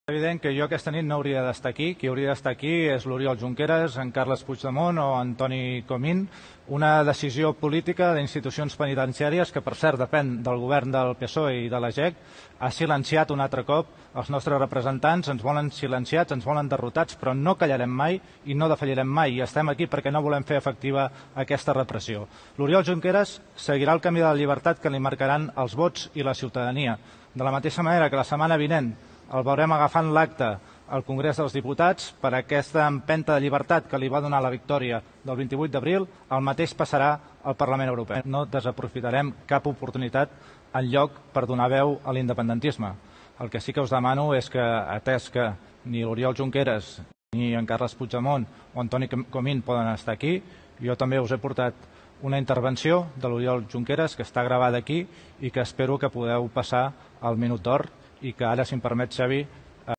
Jordi Solé @Esquerra_ERC : "Avui aquí hi haurien de ser Junqueras, Comín o el president Puigdemont.
Jordi Solé @Esquerra_ERC : "Avui aquí hi haurien de ser Junqueras, Comín o el president Puigdemont. Ens volen silenciats i derrotats, però no callarem ni defallirem mai. No volem fer efectiva la repressió. No desaprofitarem cap oportunitat per donar veu a l'independentisme 26MOmplimLesUrnes 26MFemRepública @elvellblues